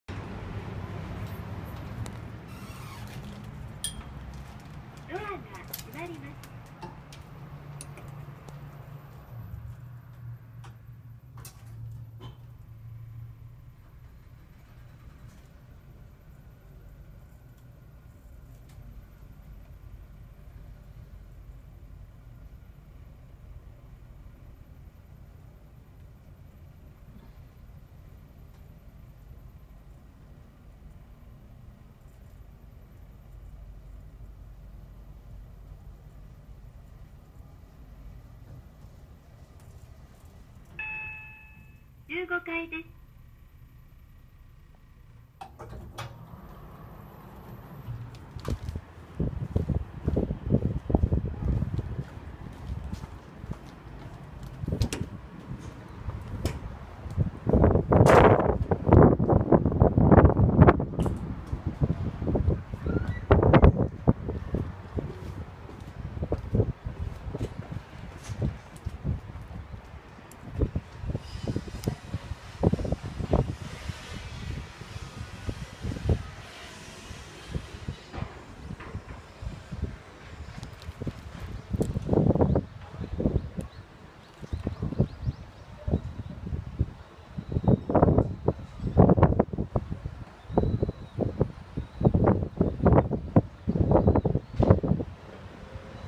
葛飾区の某マンションのエレベーター１階から１５階へ、１５階から見える景色
葛飾区にある某マンションのエレベーターです。エレベーターの速度は標準よりも速いです。最上階の１５階からの景色が、そこそこいいので最後にそれを撮りました。